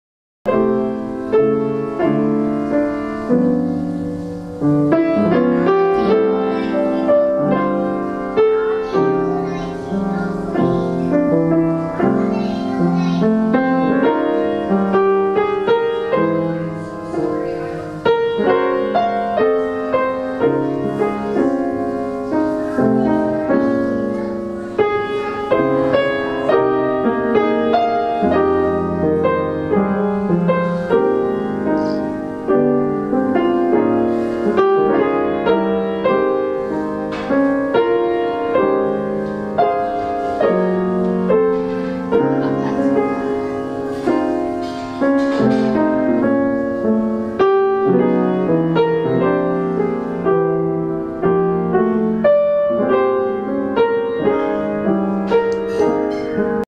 Christopher Luthi after thanksgiving dinner
My brother just messing around amongst the chaos of family. Fun times.